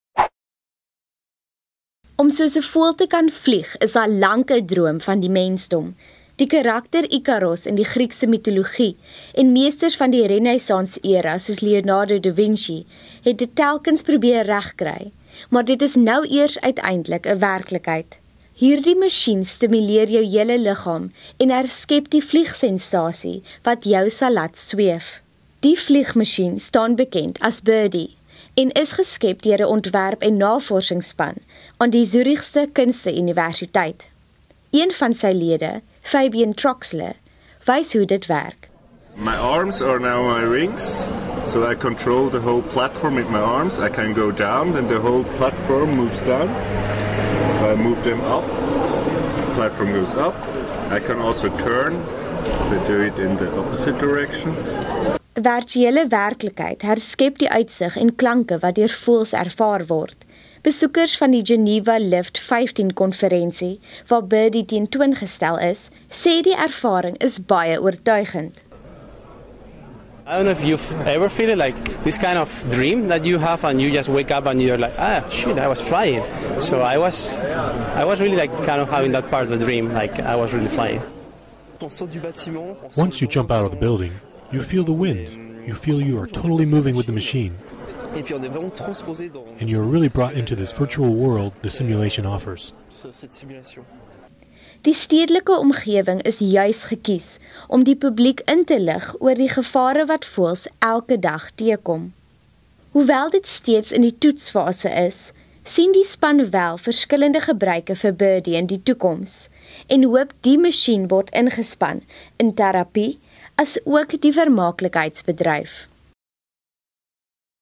‘Birdy’ gee mense vlerke
Om soos ’n voël te kan vlieg, is al lank ’n droom van die mensdom, maar dit is nou eers uiteindelik ’n werklikheid. Hierdie masjien stimuleer jou hele liggaam en herskep die vliegsensasie wat jou sal laat sweef.